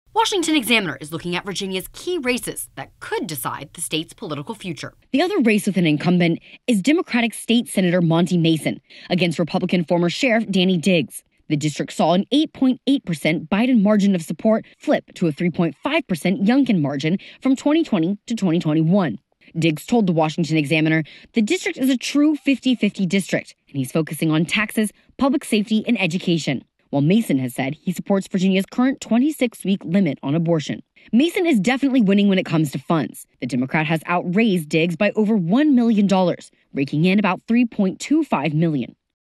Virginia Elections: Monty Mason v. Danny Diggs
The other race with an incumbent is Democratic state Sen. Monty Mason against Republican former sheriff Danny Diggs. This district saw a 8.8% Biden margin of support flip to a 3.5% Youngkin margin from 2020 to 2021.

Diggs told the Washington Examiner the district is a "true 50/50 district," and he is focusing on "taxes, public safety, and education." While Mason has said he supports Virginia's current 26-week limit on abortion. Mason is definitely winning when it comes to funds. The democrat has outraised Diggs by over $1 million, raking in about $3.25 million.